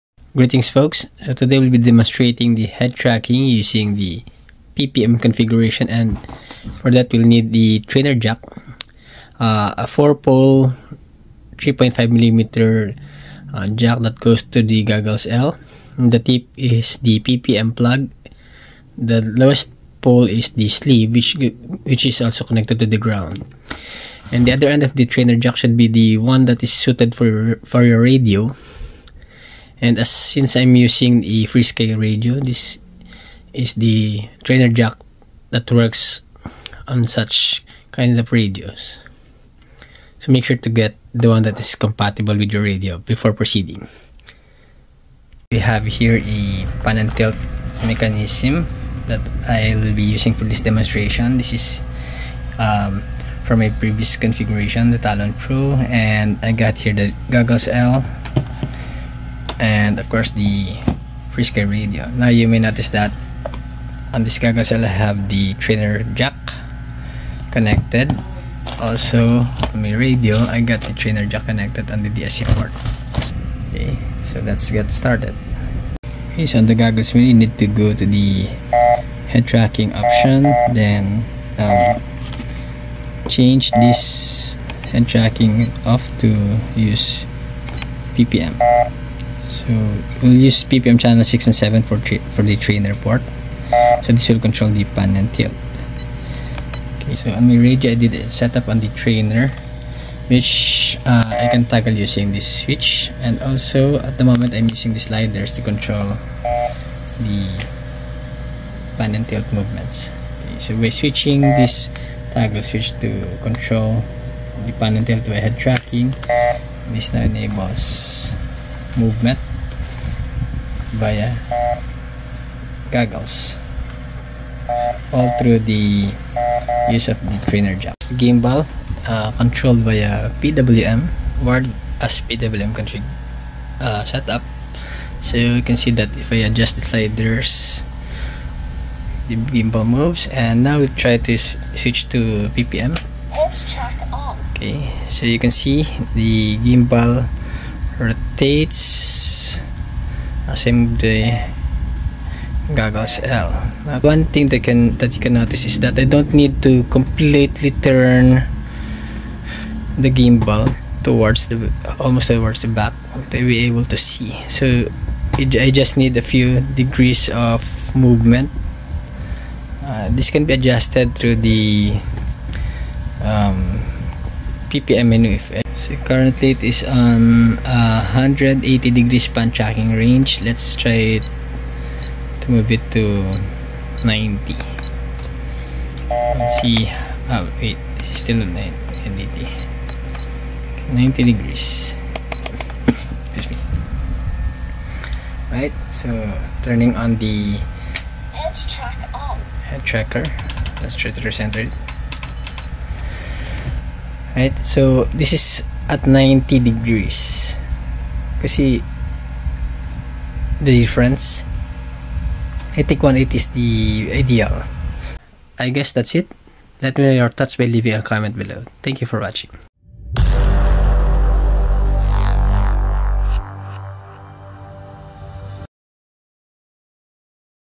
Headtracking via PPM on Walksnail Goggles L?
Here's a quick demonstration of Walksnail Goggles L head tracking via PPM (instead of using the wireless PTZ mode). For a brief background, Headtracking via PTZ mode requires a Walksnail Moonlight vtx or a Walksnail Avatar V2 vtx (need to plug the uart lines of the gimbal and moonlight /v2 vtx). If using a different Walksnail vtx such as a V1 vtx, headtracking via PTZ won't work. The solution is to use PPM Mode instead. In this configuration, the goggles L will send head movements to the radio via trainer cable, and the radio will forward these signals to the gimbal (or a servo based pan tilt mechanism) via pwm lines. You need to connect the PWM1 to PWM4 lines of the gimbal to any spare output/PWM pin on your flight controller or receiver. Then you need to configure the radio controller to have the trainer enabled (via toggle switch) and set the trainer channels accordingly. I used PWM6 and 7 for the trainer channels to control the pan and tilt respectively.